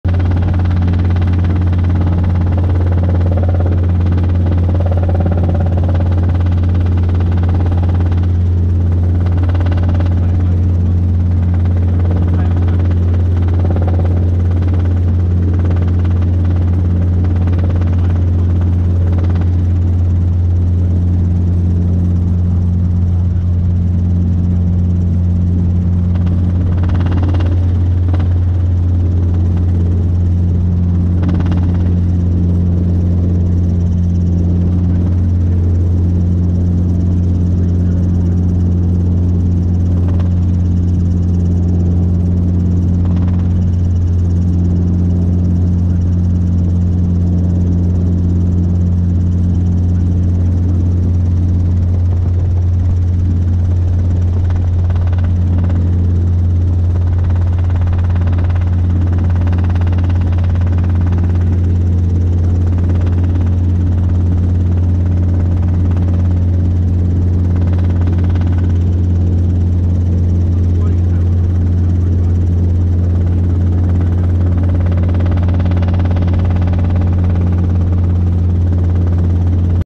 Empire state building by helicopter
helicopter robinson r22 circling empire state building checking out over Intrepid